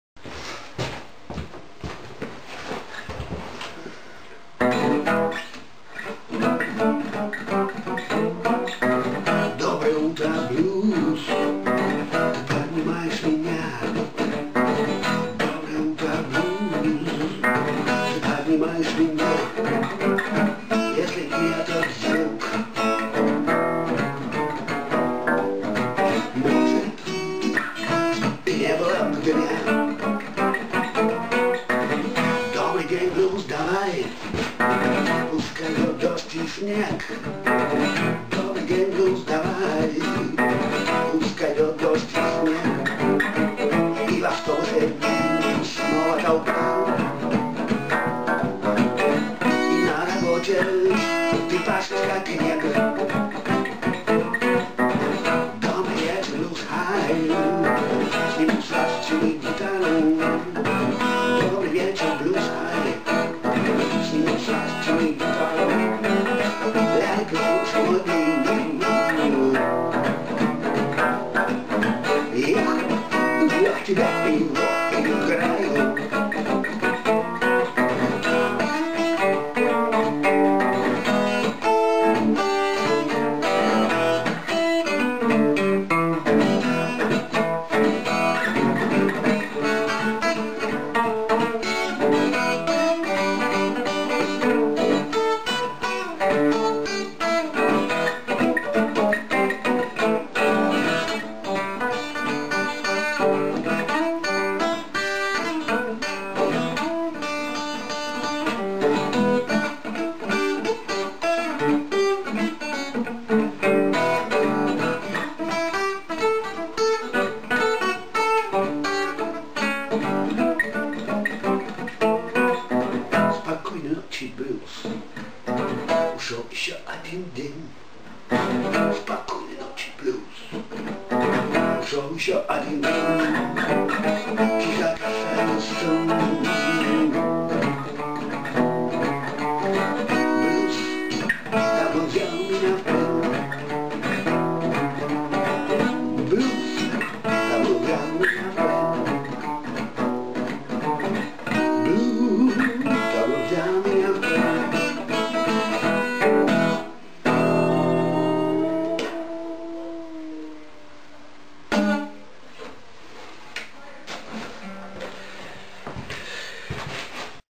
блюз о блюзе.wmv
Рабочий полдень...Попытка исполнения блюза на гитаре,обычно используемой в качестве театрального реквизита...